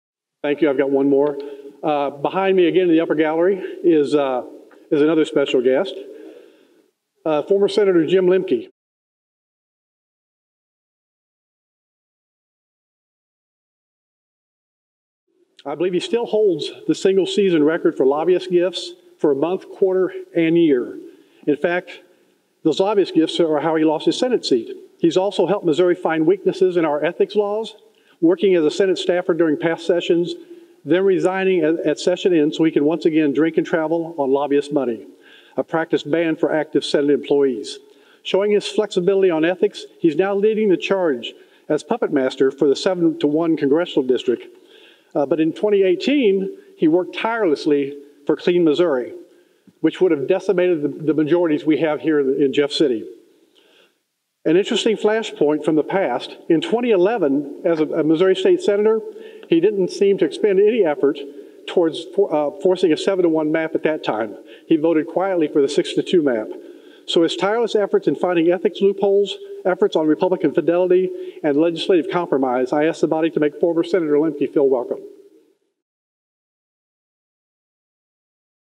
MO State Sen. Cierpiot introduces Jim Lembke
Sen. Mike Cierpiot, R-Lee's Summit, attacks former Sen. Jim Lembke on Thursday, Feb. 10, 2022. Lembke is an advisor to the conservative caucus, which is filibustering over Missouri's congressional redistricting map.